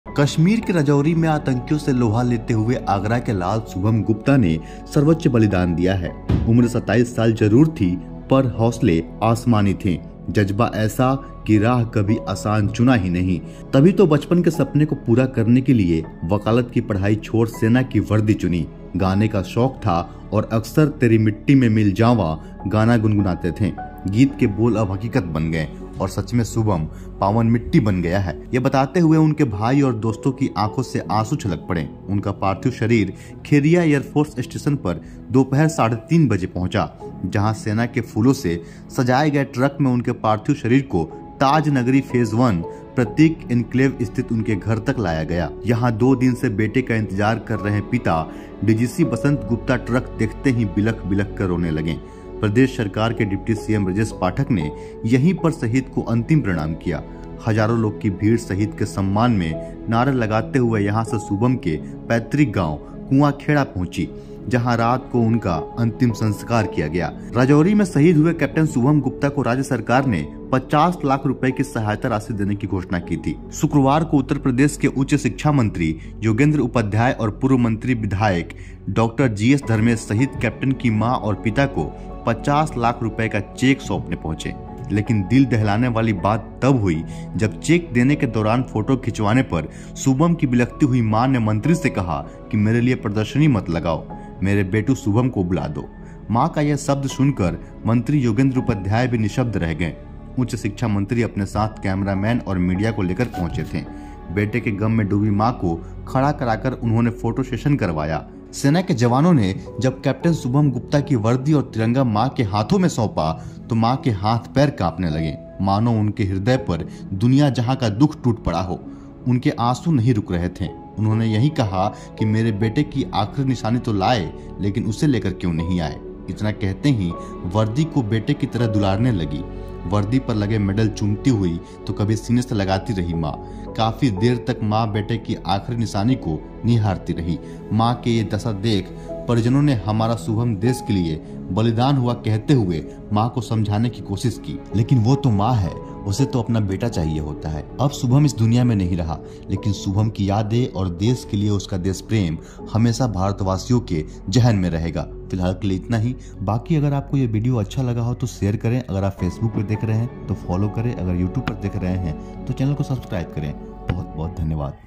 राजौरी में शहीद हुए शुभम गुप्ता की आखिरी निशानी देख बिलख उठा परिवार! | Captain Shubham Gupta
जम्मू-कश्मीर के राजौरी में आतंकियों से मुठभेड़ में कैप्टन शुभम गुप्ता शहीद हो गए। आगरा के रहने वाले शुभम गुप्ता के परिवार में मातम छाया हुआ है. शुभम की अंतिम यात्रा में भारी जनसैलाब देखने को मिला।

Captain Shubham Gupta was martyred in an encounter with terrorists in Rajouri, Jammu and Kashmir. There is mourning in the family of Shubham Gupta, a resident of Agra. A huge crowd was seen in Shubham's last journey.